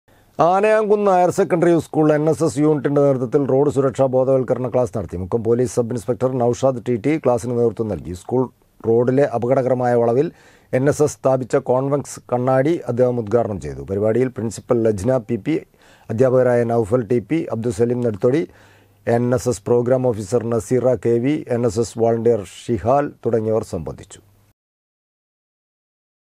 എൻ എസ് എസ് യൂണിറ്റിന്റെ നേതൃത്വത്തിൽ റോഡ് സുരക്ഷ ബോധവൽകരണ ക്ലാസ് നടത്തി
ആനയാംകുന്ന് ഹയർസെക്കണ്ടറി സ്കൂൾ എൻ എസ് എസ് യൂണിറ്റിന്റെ  നേതൃത്വത്തിൽ റോഡ് സുരക്ഷ ബോധവൽകരണ ക്ലാസ്   നടത്തി